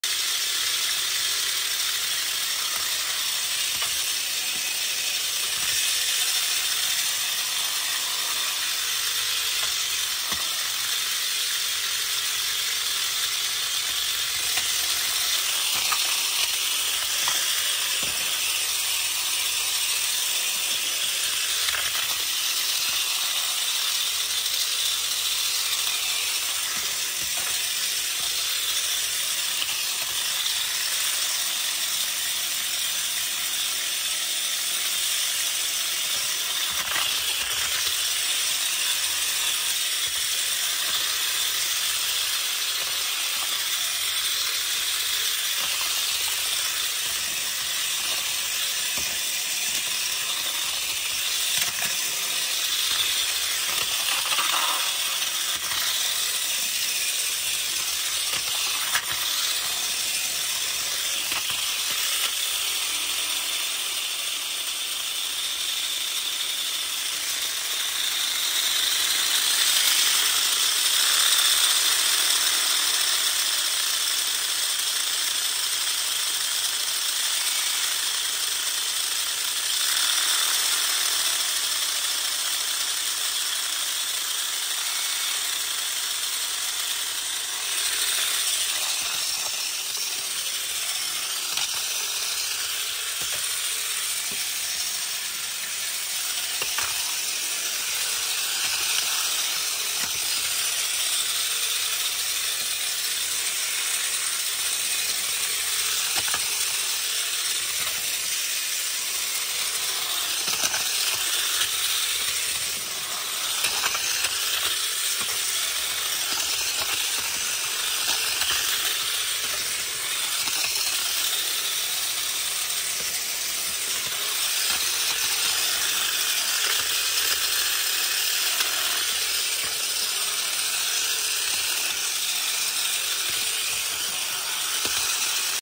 クラッシュギア テストシングルiod
壁詰まり気味のギアはシングルiodの方がいい動きをしますね。アームが片面しかないので壁からの復帰率も高いです。という訳で第三世代のiodは没収します！